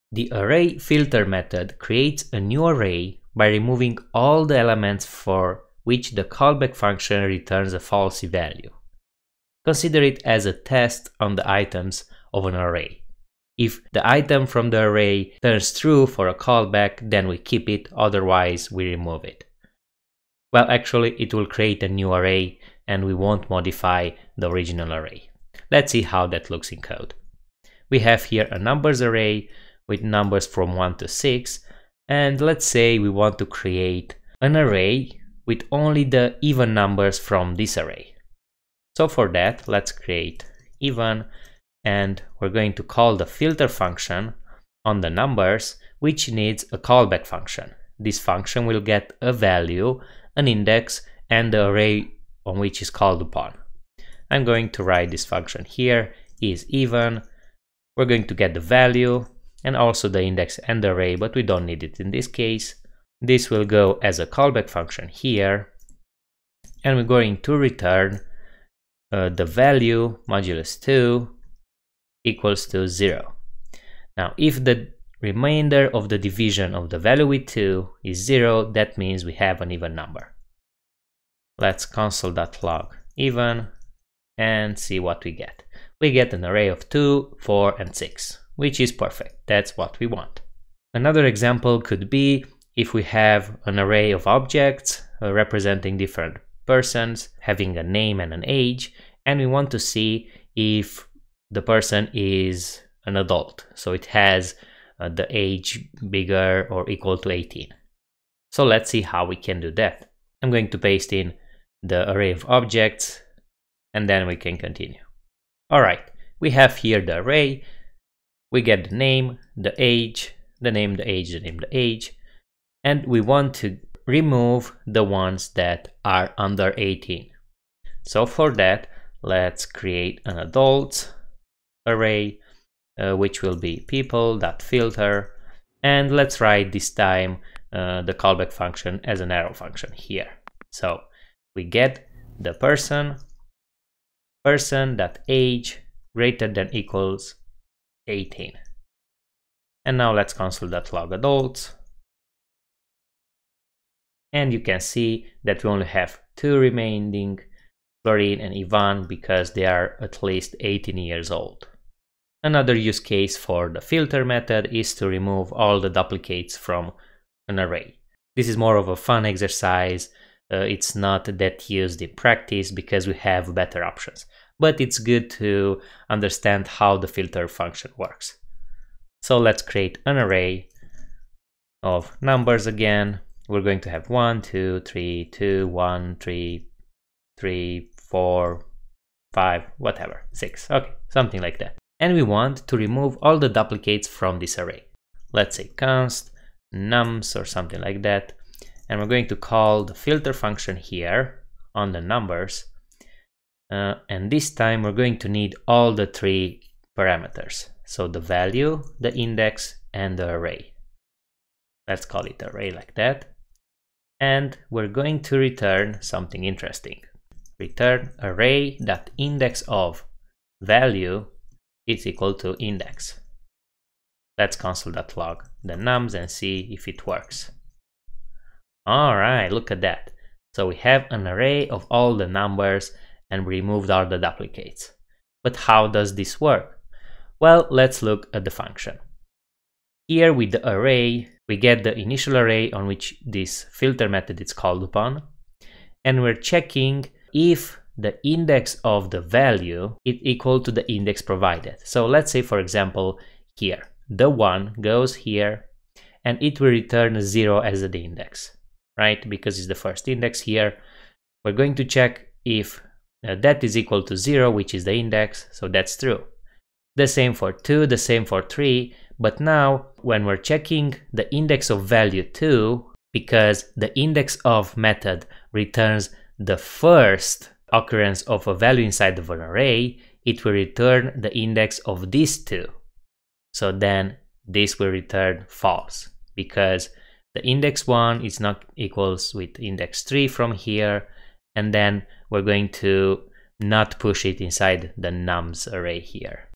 filter Array Method | JavaScript Tutorial
In this tutorial we're going to learn about the filter JavaScript Array Method and how it can be used to create a new array which will contain all the values for which the callback function returns a truthy value.